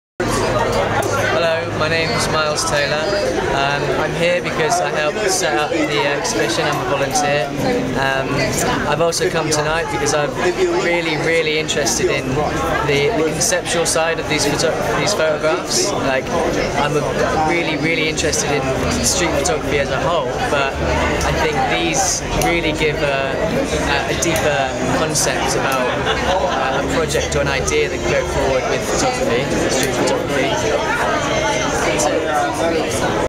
Miles Taylor talks about Seen/Unseen exhibition
Festival volunteer Miles Taylor talks about his love of street photography and why he is attending the launch night of George Georgiou and Mimi Mollica's Seen/Unseen exhibition.